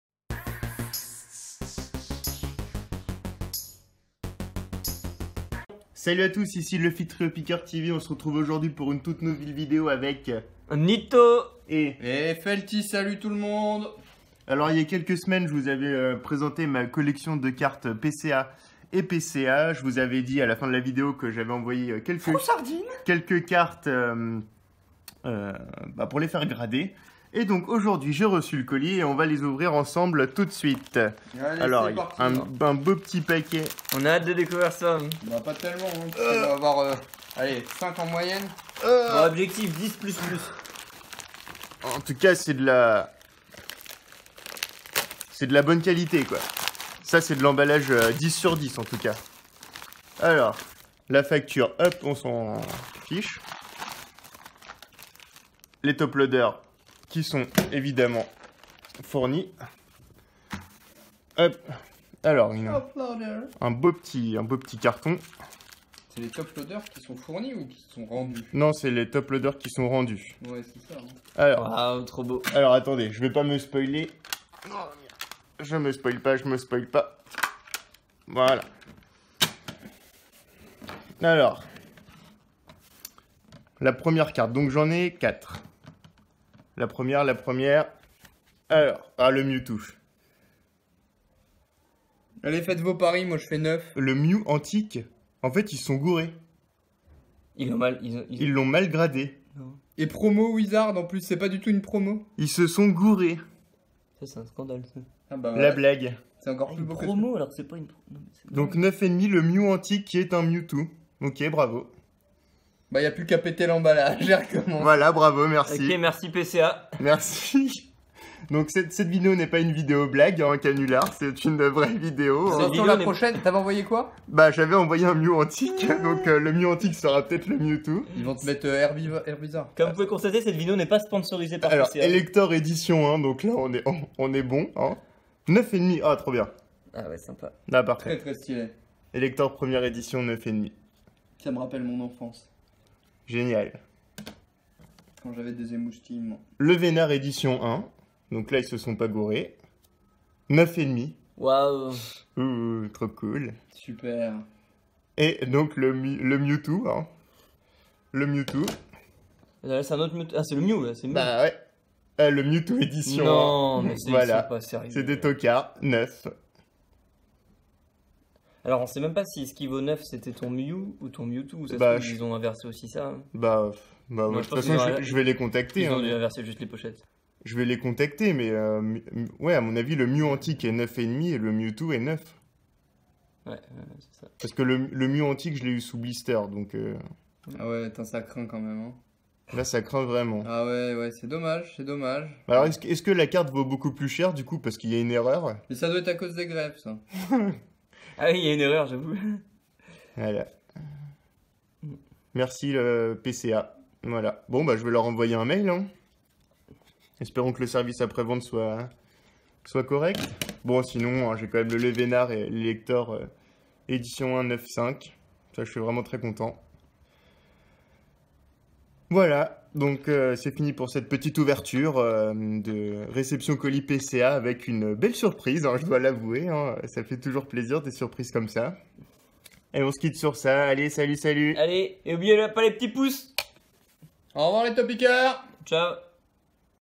Incroyable Erreur de PCA Pokémon !!!
Incroyable erreur de la part de PCA.
Les cartes valent-elles plus cher? :)

Les cartes ont depuis été changé par PCA.

Cette vidéo n’est pas là pour faire du tort à PCA. Ils font du superbe boulot. L’erreur est humaine.